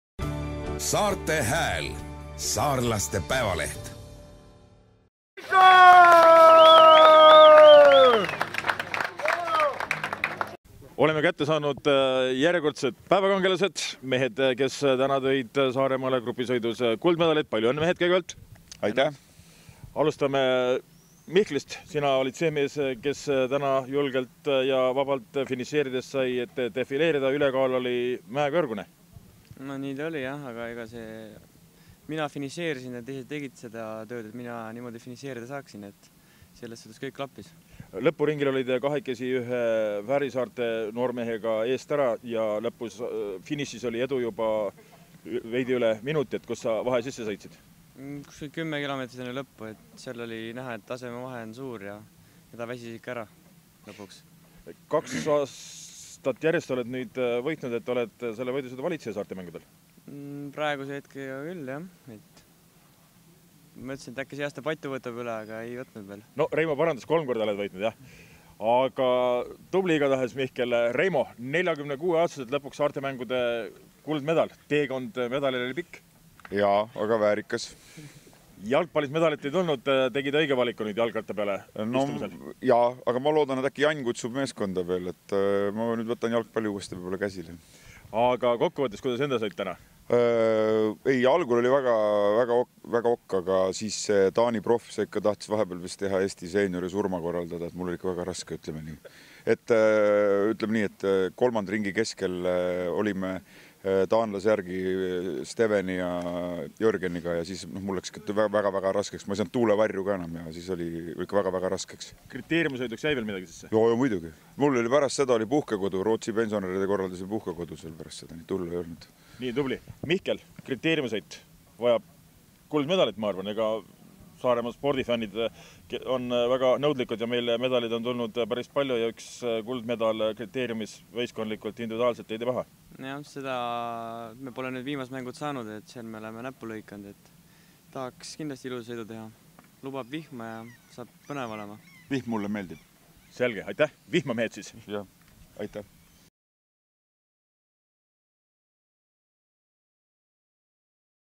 Saarte Mängud Gotlandil 2017 | jalgratta maanteesõit
Saarlane Mihkel Räim võitis Gotlandil oma kolmanda kuldmedali Saarte mängude ajaloos. Saarlased said ka meeskondliku kulla, teiste hulgas ka 46 aastane Reimo Nook.